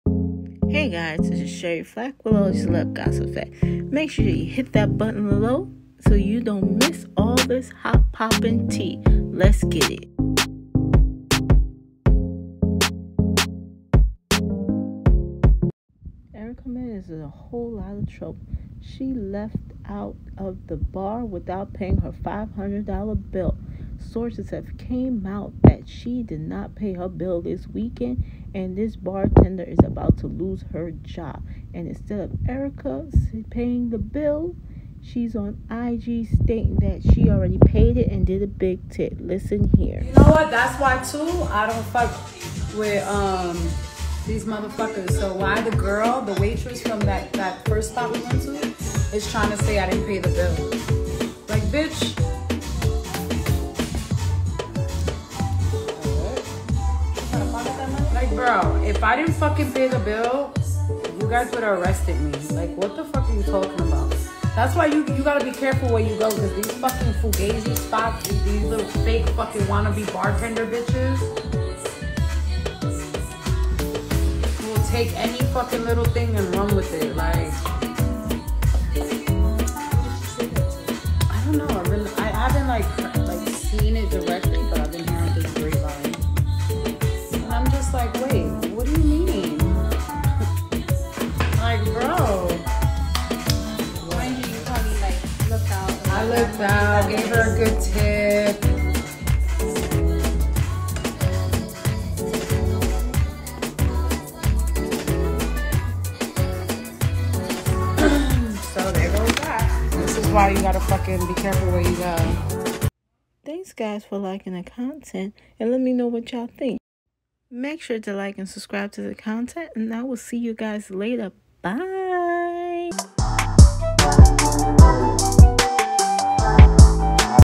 Erica Mena Denies Dine and Dash
Erica Denies Not Paying A 500 Tab At The Bar.  Find Out All The Details Here.